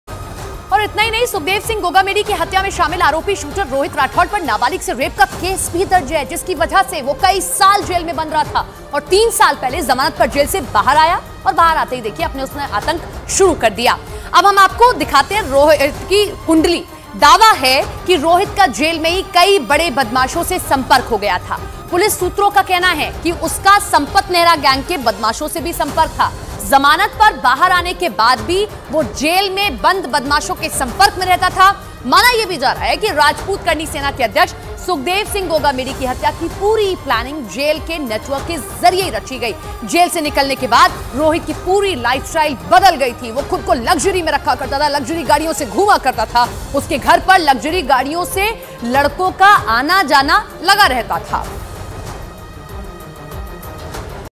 Sukhdev Singh Gogamedi Murder: आोरपी रोहित राठौड़ का पर्दाफाश,देखिए क्राइम कुंडली।Breaking News|N18V
Sukhdev Singh Gogamedi Murder: Rohit Rathod, accused in the Sukhdev Singh murder, also has a case registered for rape of a minor due to which he was in jail for many years. Terror started again as soon as he came out of jail
Sukhdev Singh Gogamedi Murder : सुखदेव सिंह हत्या में आरोपी रोहित राठौड़ नाबालिक से रेप का केस भी दर्ज है जिसकी वजह से कई साल जेल में था. जेल से बाहर आते ही फिर आतंक शुरू कर दिया.